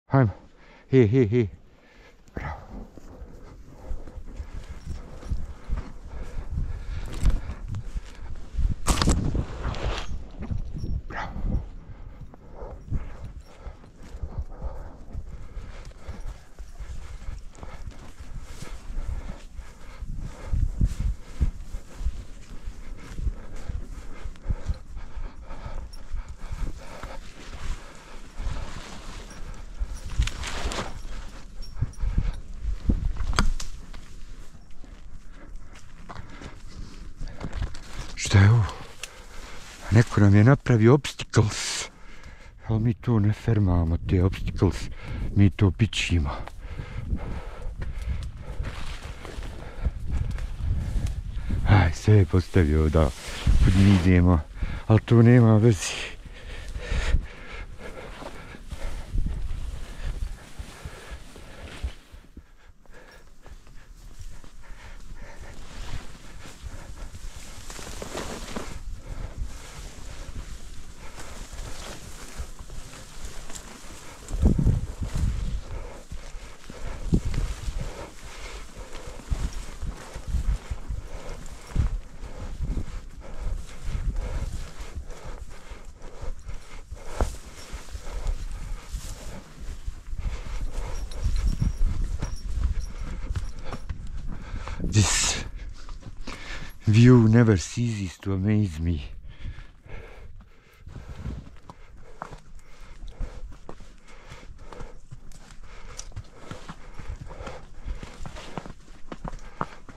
Hacking our way trough. Arabian stallion, natural horsemanship
As winter is coming and heavy snows, it will be impossible to go between or under these low branches, so I brought my ax with me. As if it isn't difficult enough somebody placed all the branches from a cut tree on our path. Bitless, bareback, barefoot.